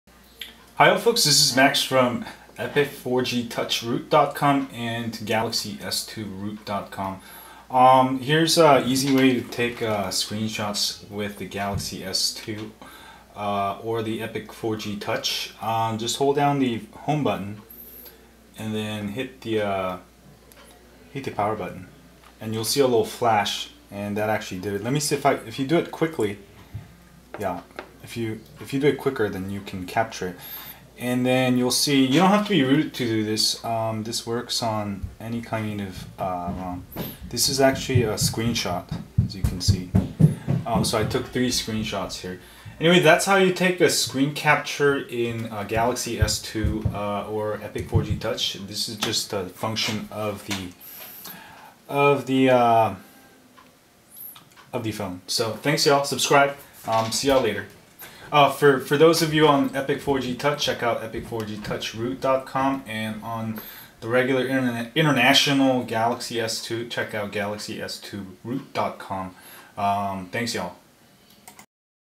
How to Screen Capture on Galaxy S2 or Epic 4G Touch!
The Galaxy S2 and Epic 4G Touch come with screen capture feature (no rooted required), just press home button and power button together.

Want to stay updated on all Epic 4G Touch Root tips and hacks?